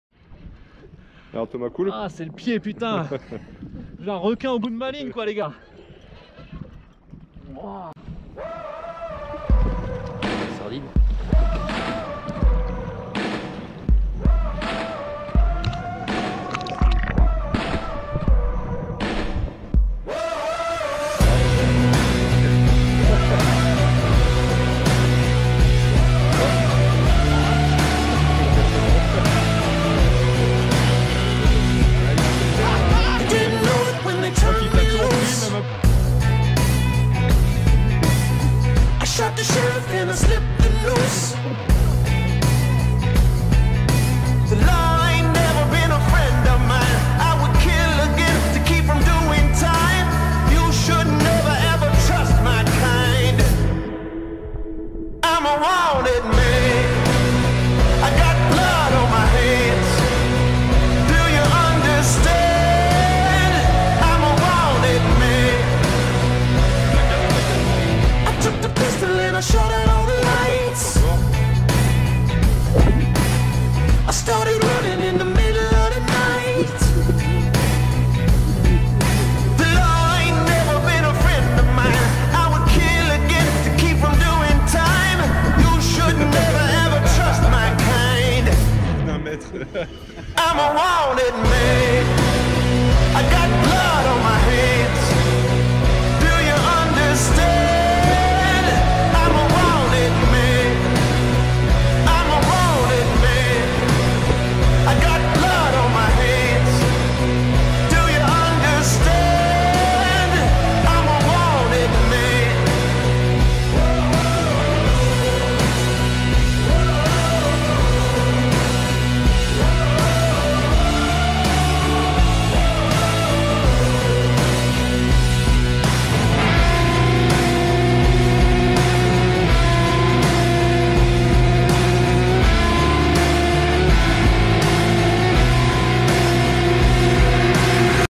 Pêche requin shark fishing Arcachon
Sortie pêche au requin, fascinant et souvent méconnu, faite connaissance avec les seigneurs des océans.